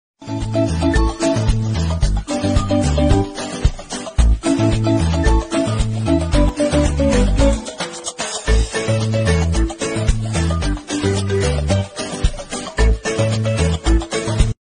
The giant panda will open the door
The giant panda will open the door. They say that the giant panda is disguised by Sichuan people. Is this evidence?